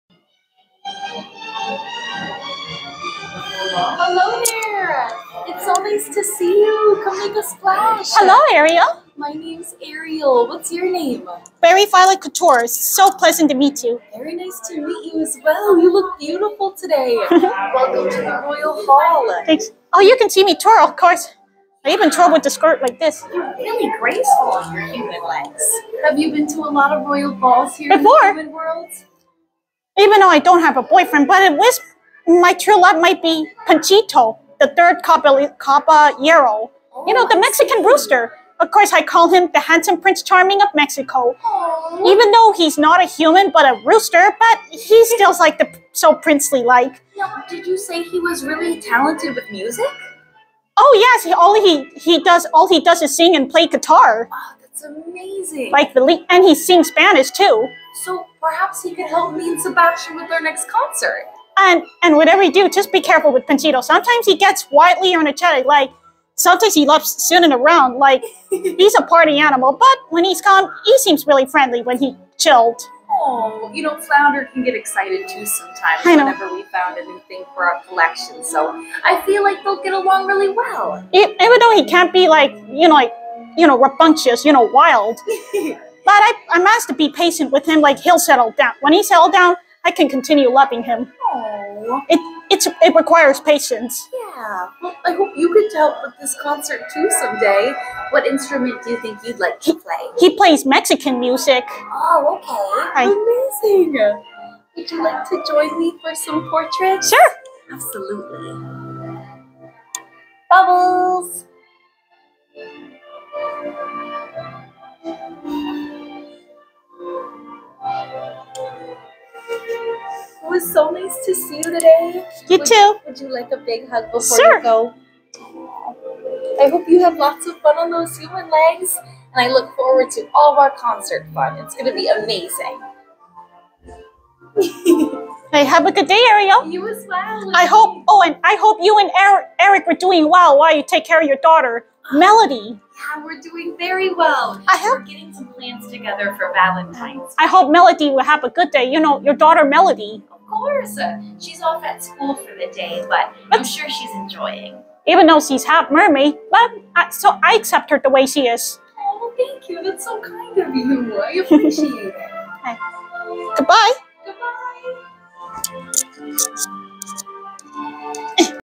Platinum Princess Berry Violet meets Ariel at Disneyland 2023
The second Disney princess in Disneyland's Royal Hall of Princesses is Ariel.

Recorded and published on Monday, February 6, 2023 at 9:05 AM

This is the sneak peek of the Disneyland Adventure blog series episode, "Princess Berry Violet's Final Day of Disneyland."

On Monday, February 6, 2023, I, dressed up in a white formal princess dress attire and a princess tiara or crown went by "Princess Berry Violet," arrived for the final day of my annual pass that will be expired in the next day. I greet, meet and interact with Mickey Mouse, Minnie Mouse, Pluto, Goofy, Daisy Duck, Donald Duck, Chip and Dale during Disney 100 Years of Wonder theme park event. This final day before the annual pass' expiration date next day is where I will miss everybody in Disneyland and I promise to come back with a renewed annual pass next year of 2024.

I appeared as the platinum princess for Disneyland 100th anniversary event occasion. Especially for Disney100 Years of Wonder.

Meet and greet Disneyland characters are present and will appear in this vlog or blog episode are:

• Mickey Mouse

• Minnie Mouse

• Goofy

• Pluto the pet dog of Mickey Mouse

• Donald Duck

• Daisy Duck

• Chip and Dale

• Geppetto from Disney "Pinocchio"

• Cinderella

• Snow White

• Ariel from "The Little Mermaid"

• Woody, Jessie the Yodeling Cowgirl, Bo Peep and Buzz Lightyear from Disney Pixar "Toy Story"

• Joy and Sadness from Pixar's "Inside Out"

• Miguel Rivera from Disney Pixar "Coco"

Location at both Disneyland Resort Park at Anaheim, California and Disney California Adventure.